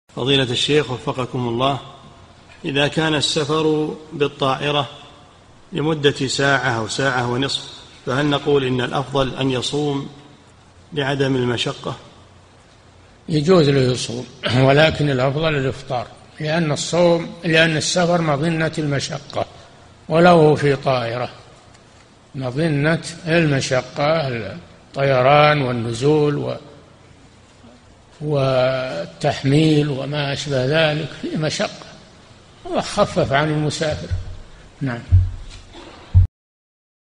[1097 -3022] إذا كان السفر بالطائرة لا يشق على المسافر فهل نقول إن الأفضل أن يصوم لعدم المشقة؟
-"[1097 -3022] السؤال: إذا كان السفر بالطائرة لمدة ساعة أو ساعة ونصف فهل نقول إن الأفضل أن يصوم لعدم المشقة؟
الجواب: الجَوَابُ: يجوز له يصوم ولكن الأفضل الإفطار لأن السفر مظنة المشقة ولو في طائرة مظنة المشقة الطيران والنزول والتحميل وما أشبه ذلك فيه مشقة، الله خفف عن المسافر، نعم.
للاشتراك في قناة الشيخ صالح الفوزان

للاستفسار والإقتراحات والدعم
رئيس الفريق
لمشروع كبار العلماء